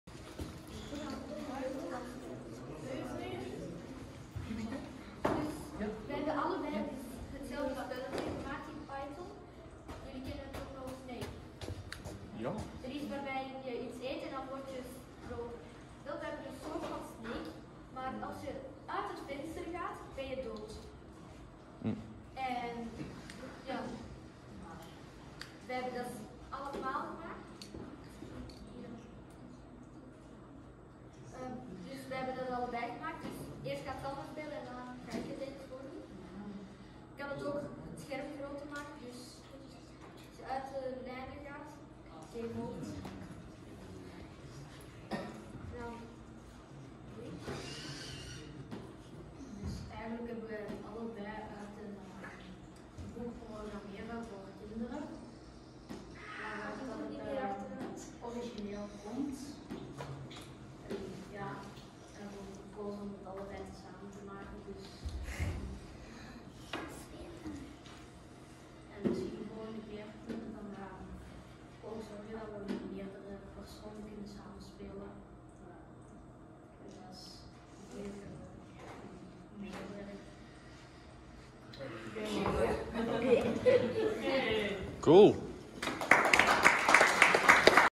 Mijn oudste zoon presenteert zijn spelletje snake gebouwd in Python
Tijdens de laatste CoderDojo heeft Xander en zijn vriendje Sander hun eerste snake game gepresenteerd dat was gebouwd door hen in Python.